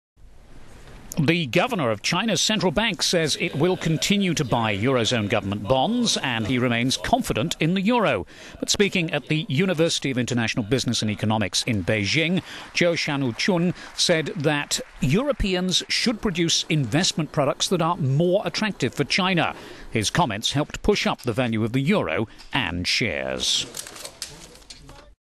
China's eurozone vote of confidence
The governor of China's central bank has said it will continue to buy eurozone government bonds and he remains confident in the euro.

But speaking at the University of International Business and Economics in Beijing, Zhou Xiaochua said Europeans should produce investment products that are more attractive for China.

His comments helped push up the value of the euro and shares.